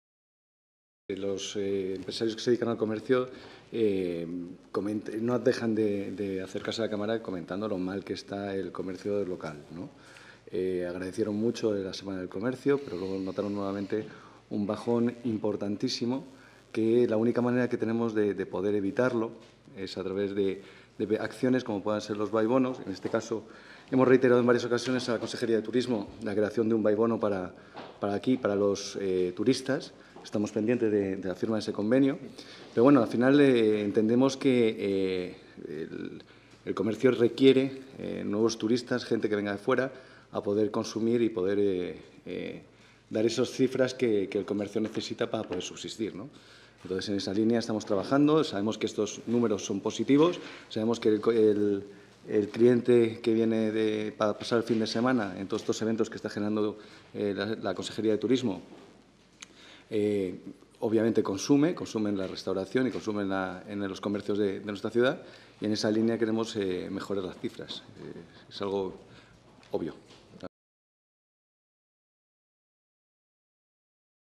La Cámara de Comercio propone buy bonos para turistas
Buy bonos para turistas. Esta es la propuesta que ha lanzado este miércoles el presidente de la Cámara de Comercio. Y es que Karim Bulaix ha vuelto a reiterar la "mala situación" que, a su juicio, están atravesando los comerciantes locales.